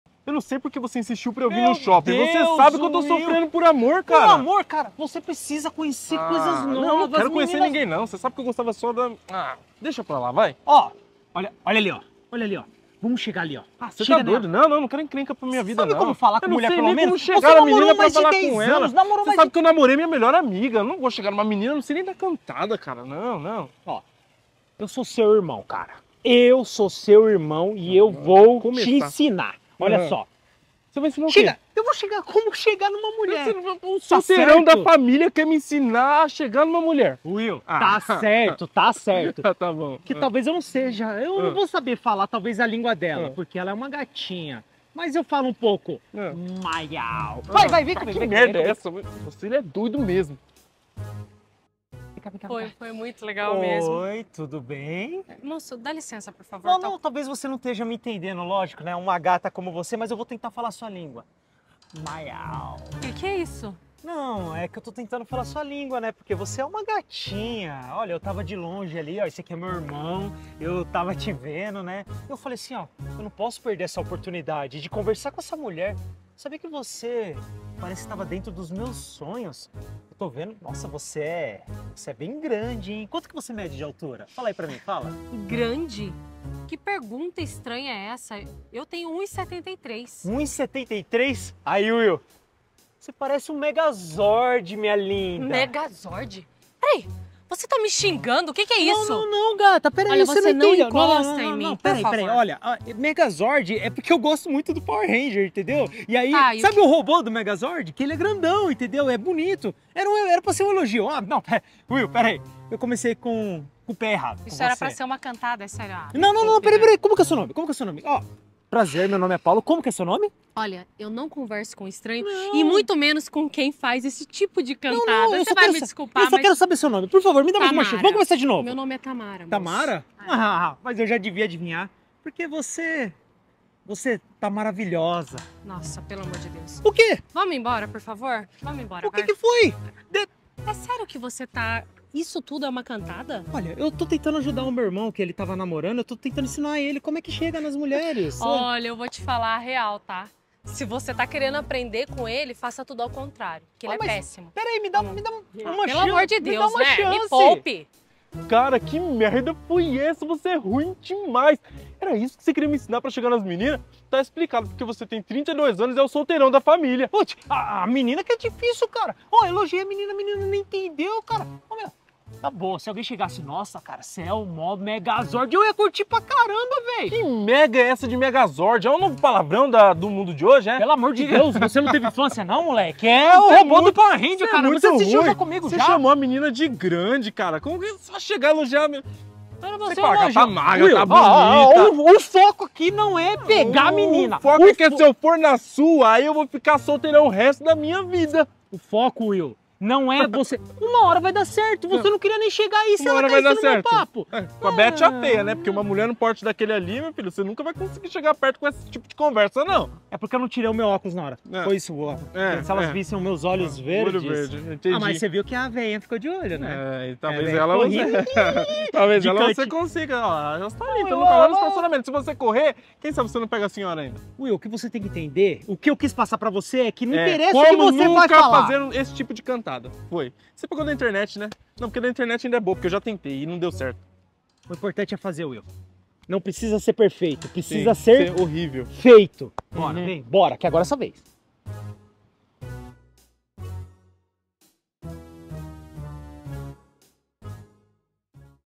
As melhores cantadas do meu irmão!
Meu irmão está tentando me ensinar como chegar em uma garota! Mas eu acho que não é assim!